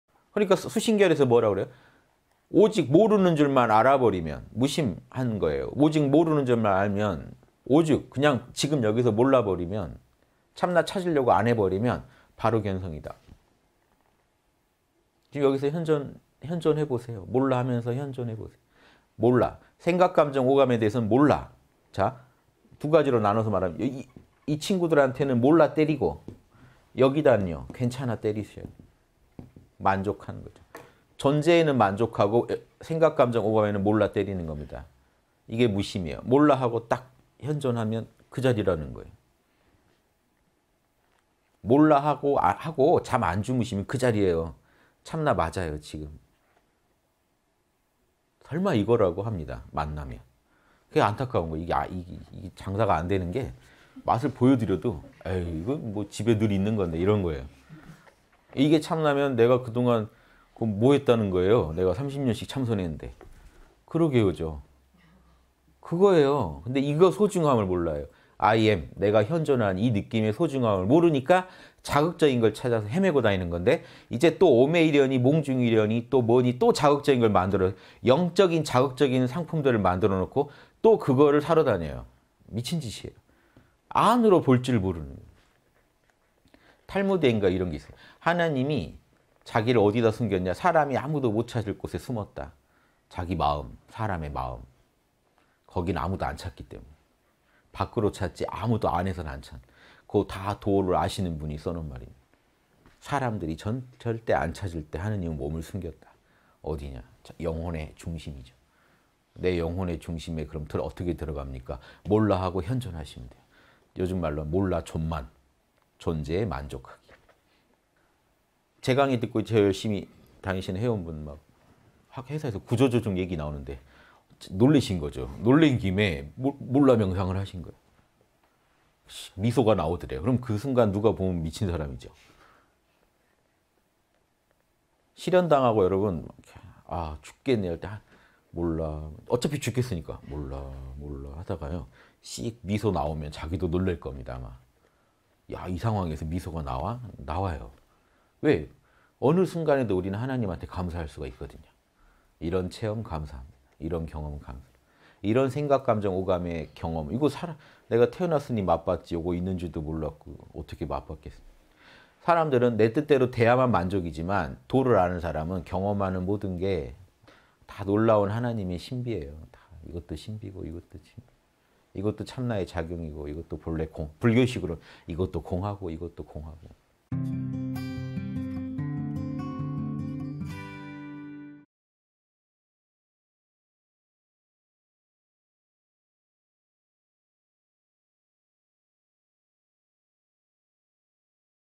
[3분 법문] 단지불회但知不會 시즉견성是卽見性
(강의 中) 내가 현존하는 이 느낌의 소중함을 모르니까, 자극적인 걸 찾아서 헤매고 다니는 건데, 이제 또 오매일여(寤寐一如)니 몽중일여(夢中一如)니 또 뭐니, 또 자극적인 걸 만들어서
영적인 자극적인 상품들을 만들어 놓고, 또 그것을 사러 다녀요. 미친 짓이에요, 안으로 볼 줄 모르는.

-200212 수낮 대담 에서 발췌함-

그러니까 『수심결 강의』에서 뭐라고 그래요?
오직 모르는 줄만 알아버리면 '무심(無心)'한 거예요.
오직 모르는 줄만 알면,
오직 그냥 지금 여기서 "몰라!" 버리면,
참나 찾으려고 안 해버리면 바로 견성이다.
지금 여기서 현존해 보세요.
"몰라!" 하면서 현존해 보세요.

"몰라!" 생각ㆍ감정ㆍ오감에 대해서는 "몰라!",
자, 두 가지로 나눠서 말할 게요.
이 친구들한테는 "몰라!" 때리고
여기다는 "괜찮아!" 때리세요. 만족하는 거죠.
존재에는 만족하고 생각ㆍ감정ㆍ오감에는
"몰라!" 때리는 겁니다. 이게 무심이에요.
"몰라!" 하고 딱 현존하면 그 자리라는 거예요.

"몰라!" 하고 잠 안 주무시면 그 자리예요.
참나 맞아요, 지금. "설마 이거라고?" 합니다, 만나면.
그게 안타까운 거예요.
이게 장사가 안 되는 게, 맛을 보여드려도
"이거 뭐 집에 늘 있는 건데." 이런 거예요.
"이게 참나면 내가 그동안 그럼 뭐 했다는 거예요?
내가 30년씩 참선했는데!"
"그러게요."죠. 그거예요.
그런데 이거 소중함을 몰라요.

'I AM' 내가 현존하는 이 느낌의 소중함을 모르니까,
자극적인 걸 찾아서 헤매고 다니는 건데,
이제 또 오매일여(寤寐一如)니 몽중일여(夢中一如)니
또 뭐니, 또 자극적인 걸 만들어서
영적인 자극적인 상품들을 만들어 놓고,
또 그것을 사러 다녀요.
미친 짓이에요, 안으로 볼 줄 모르는.

『탈무드』엔가 이런 게 있어요.
"하나님이 자기를 어디다 숨겼냐?"
"사람이 아무도 못 찾을 곳에 숨었다."
자기 마음, 사람의 마음,
거긴 아무도 안 찾기 때문에,
밖으로 찾지 아무도 안에서 안 찾아요.
그거 다 도(道)를 아시는 분이 써놓은 말입니다.

사람들이 절대 안 찾을 데에 하나님은 몸을 숨겼다.
어디냐? 영혼의 중심이죠.
"내 영혼의 중심에 그럼 어떻게 들어갑니까?"
"몰라!" 하고 현존하시면 돼요.
요즘 말로 하면, "몰라! 존만!" "존재에 만족하기"
제 강의 듣고 열심히 다니시는 회원분,
회사에서 구조조정 얘기 나오는데, 놀라신 거죠.

놀란 김에 "몰라!" 명상을 하신 거예요.
씩 미소가 나오더래요.
그러면 그 순간 누가 보면 미친 사람이죠.
실연당하고 여러분 "아, 죽겠네!" 할 때,
"몰라!" 어차피 죽겠으니까, "몰라!" "몰라!" 하다가요,
씩 미소 나오면 자기도 놀랄 겁니다, 아마.
"야, 이 상황에서 미소가 나와?", 나와요.

왜? 어느 순간에도 우리는 하나님한테 감사할 수가 있거든요.
"이런 체험 감사합니다. 이런 경험 감사합니다."
이런 생각ㆍ감정ㆍ오감의 경험,
이거 내가 태어났으니 맛봤지 이거 있는 줄도 몰랐고,
어떻게 맛봤겠어요.

사람들은 내 뜻대로 되어야만 만족이지만,
도를 아는 사람은 경험하는 모든 게
다 놀라운 하나님의 신비예요.
다 이것도 신비고, 이것도 참나의 작용이고,
이것도 본래 공(空),
불교식으로 이것도 공하고, 이것도 공합니다.